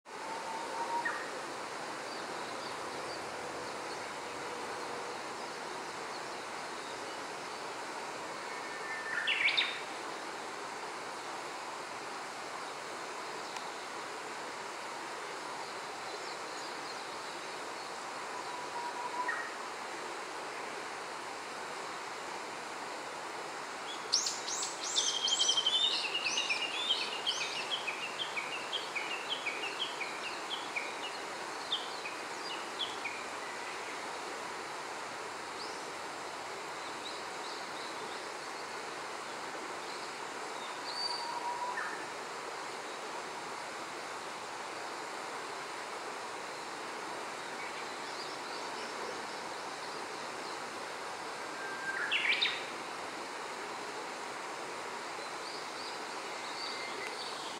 マミジロ(♂) Siberian thrush(眉白、Turdus sibiricus)
菊水山で出逢ったマミジロです。
初めて観ました。
この日は残念ながら薄曇りだったんですが、太陽光が当たるとダークブルーが煌めいて美しいと思います。
川を挟んで１０ｍぐらい離れて撮影しています。